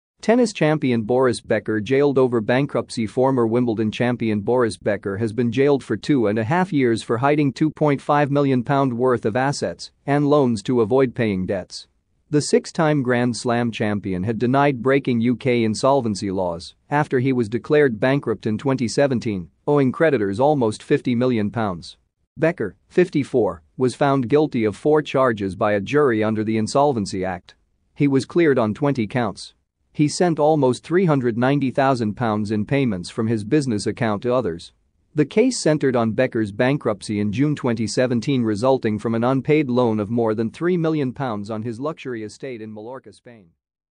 Tennis champion Boris Becker jailed over bankruptcy
Tennis champion Boris Becker jailed over bankruptcy  Former Wimbledon champion Boris Becker has been jailed for two and a half years for hiding £2.5m worth of assets and loans to avoid paying debts.  The six-time Grand Slam champion had denied breaking UK insolvency laws after he was declared bankrupt in 2017, owing creditors almost £50m.  Becker, 54, was found guilty of four charges by a jury under the Insolvency Act. He was cleared on 20 counts.  He sent almost £390,000 in payments from his business account to others.  The case centred on Becker's bankruptcy in June 2017 resulting from an unpaid loan of more than £3m on his luxury estate in Mallorca, Spain.